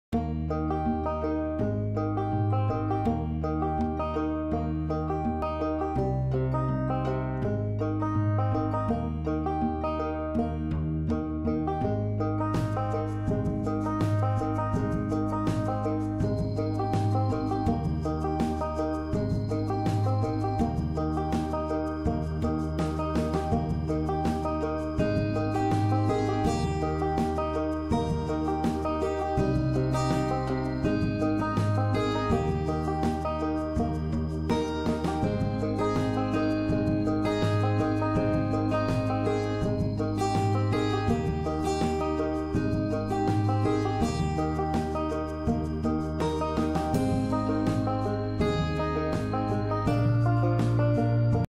instant pot split pea soup jumprope youtube
Instant Pot Split Pea Soup is an easy and classic recipe made of split peas (of course!), carrots, celery, onions, garlic, ham and a leftover ham bone for extra depth of flavor! The broth turns out rich, thick and so flavorful! You can add in potatoes too! This recipe also works well on the stove top and in a crock pot.